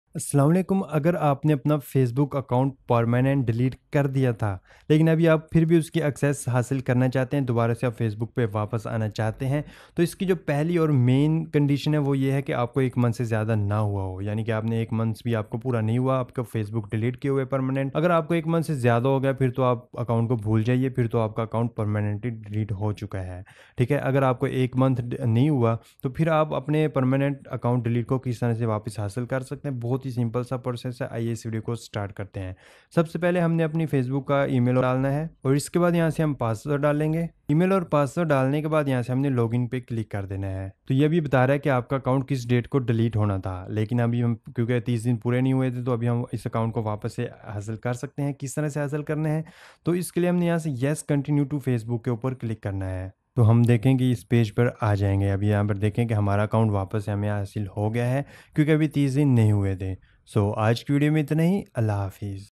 How to Recover a Permanently Deleted Facebook Account | Quick Guide
Wondering if you can recover a permanently deleted Facebook account? Unfortunately, once an account is permanently deleted, it cannot be restored. However, in this video, we'll explain the difference between deactivating and permanently deleting your account, and provide tips on how to ensure you don't lose your account in the future.

In this video, we will cover:

The difference between deactivating and permanently deleting a Facebook account
Steps to take before permanently deleting your account
Facebook's policy on account recovery
Tips to keep your Facebook account secure and avoid accidental deletion
While recovering a permanently deleted account is not possible, this guide will help you understand your options and take preventive measures for the future.